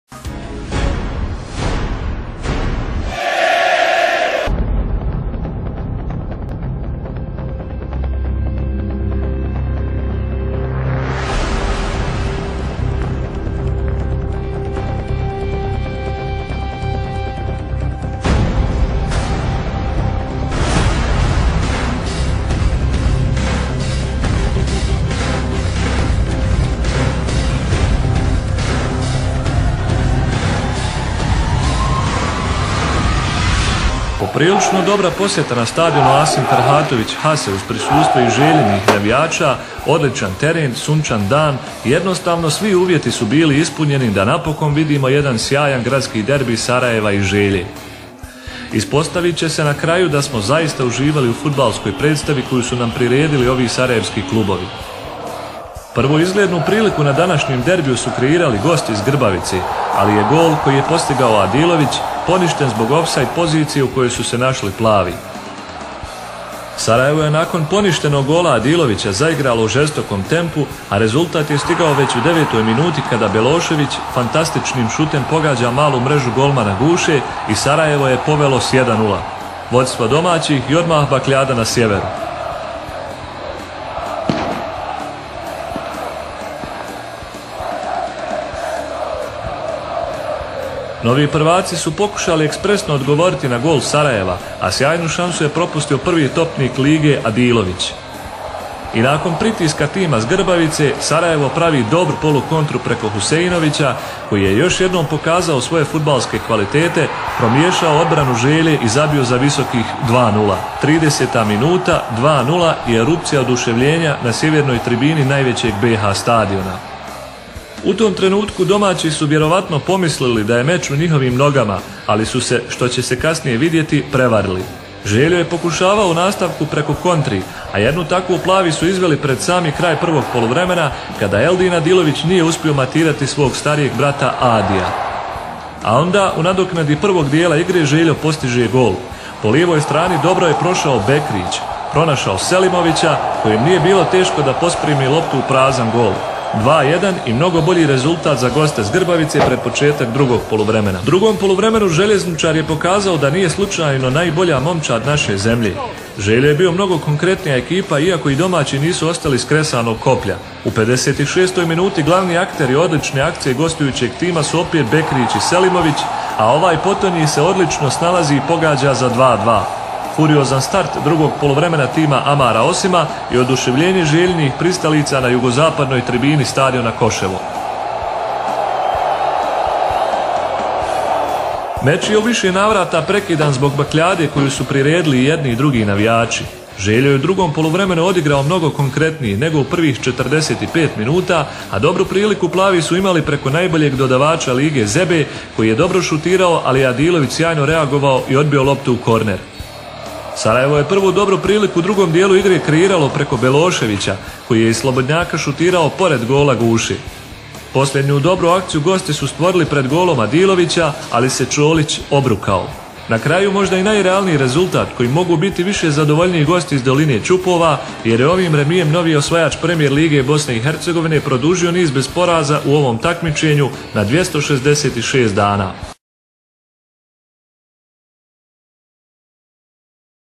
Sarajevo - Zeljeznicar 2-2
web:  fkzeljeznicar.ba
video product by: source.ba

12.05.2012.godine u Sarajevu na stadionu Asim Ferhatović Hase na Koševu pred oko 13.000 gledalaca odigran je 99.Sarajevski derbi između Sarajeva i Željezničara. Utakmica je završena neriješeno 2-2. Strelci su bili: 1-0 Belošević (9), 2-0 Huseinović (30), 2-1 Selimović (45), 2-2 Selimović (56).